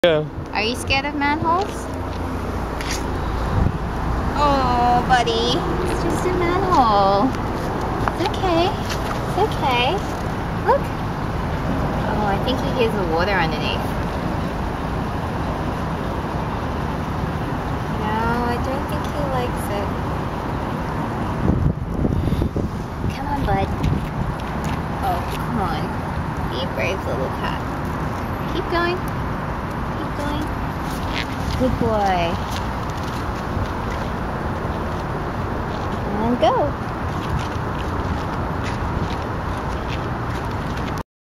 The Chronicles of Zeke: Cat vs Manhole
In the last six weeks teaching Zeke how to walk, we've found out that Zeke does not like a lot of things.  This list apparently includes manholes, which we only just found out this week.